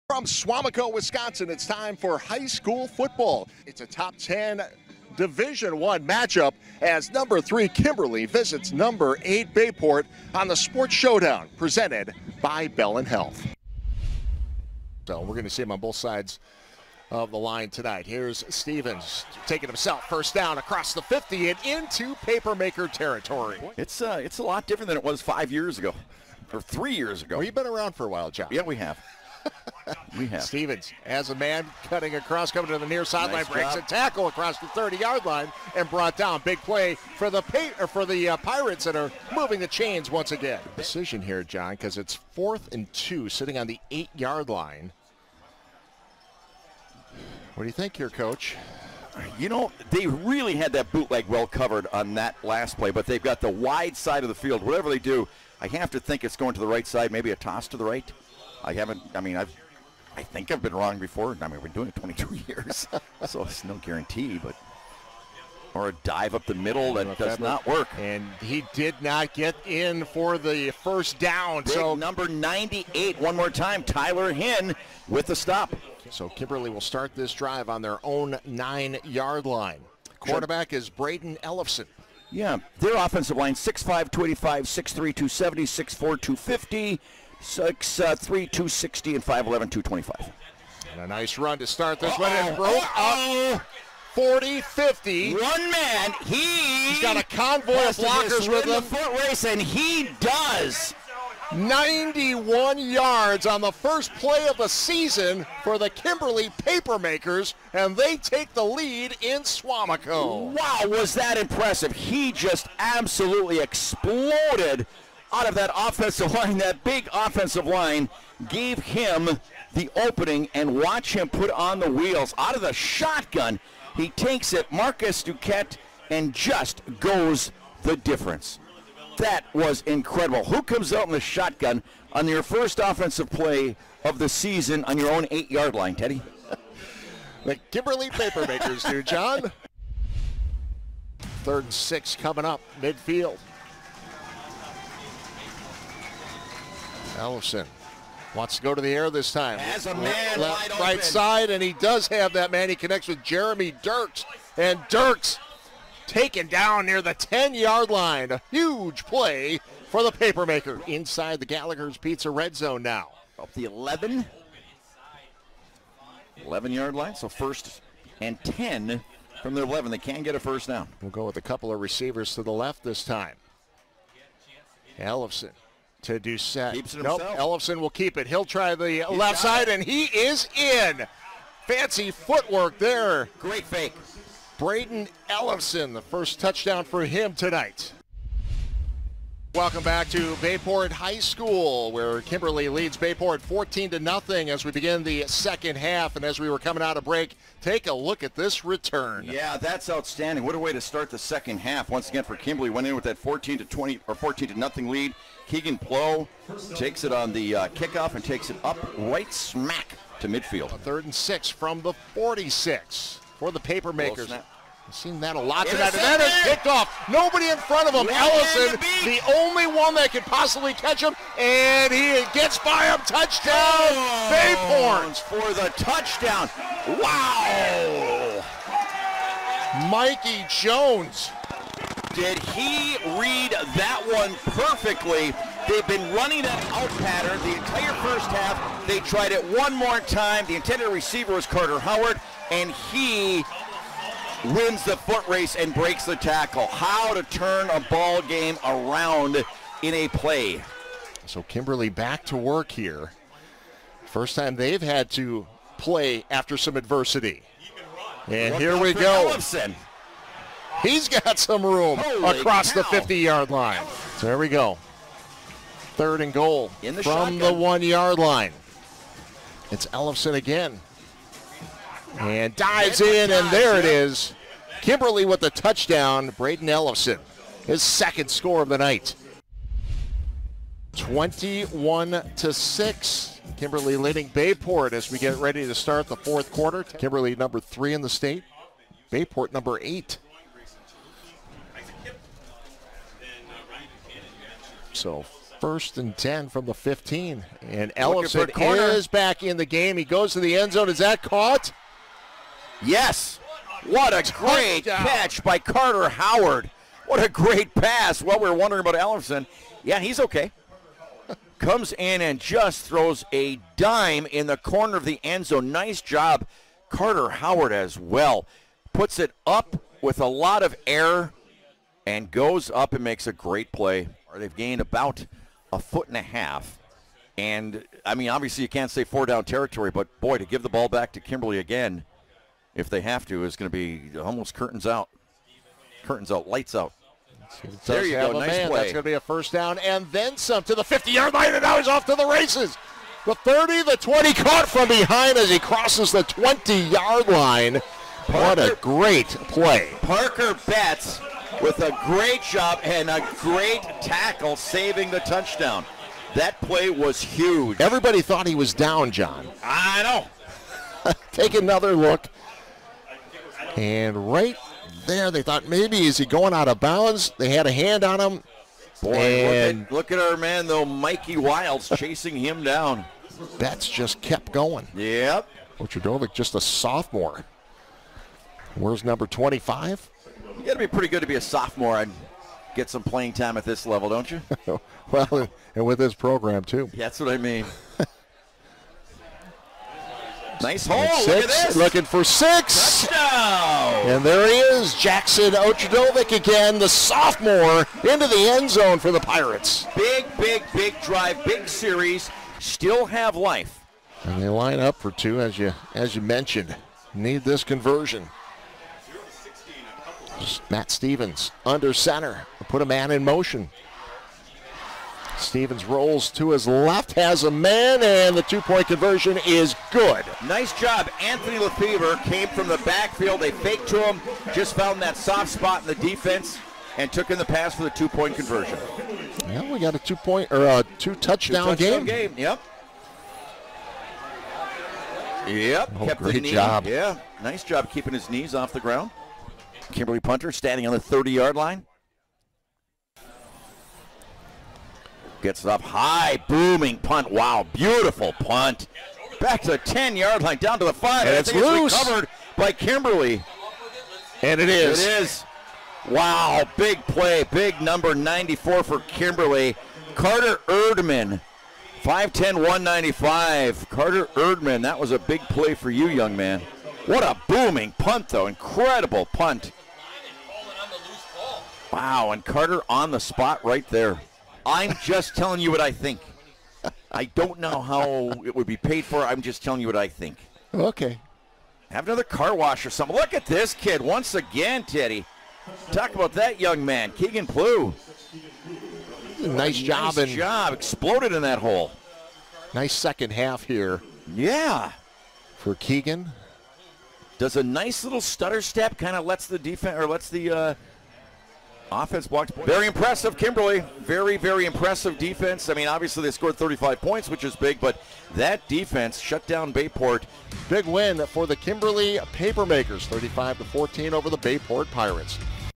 Sports Showdown Week 2 Highlights: Kimberly Papermakers vs. Bay Port Pirates
After having a bye in Week 1, Kimberly started their season on Thursday with a convincing 35-14 win over Bay Port.